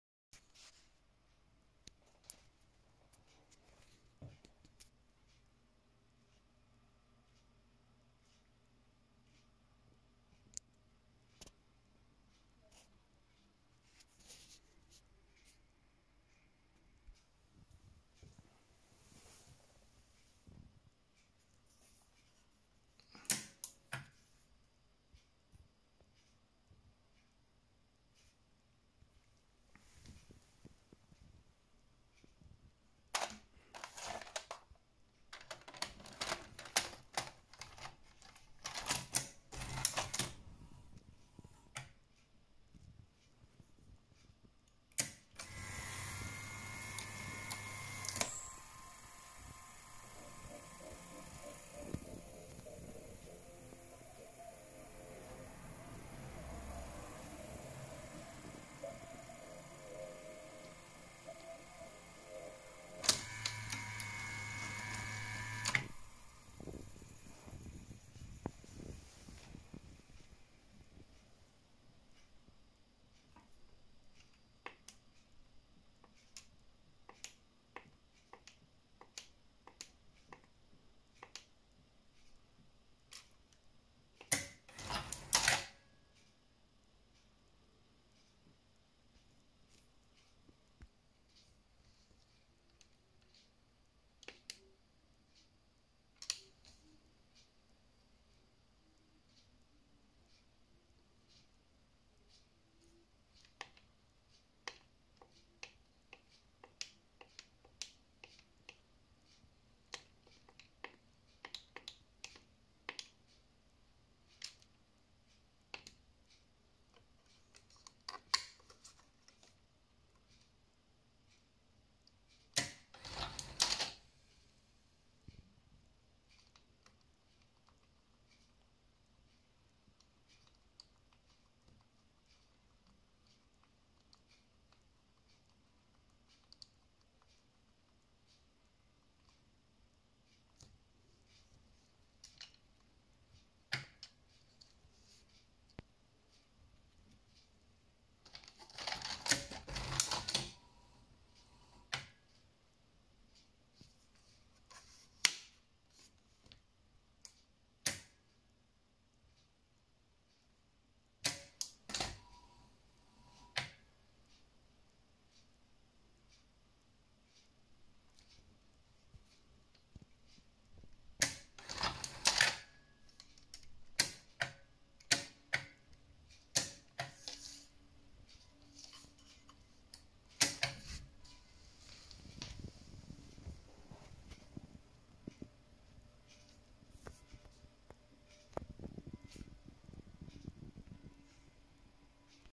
V2000 Grundig 2200 Test der reparierten Ablaufsteuerung / SQC test
Test der reparierten Ablaufsteuerung (Uhrzeit, Datum, Timer, Laufwerksfunktionen, ...)
Test of the repaired sequence control (time, date, timer, deck functions, ...)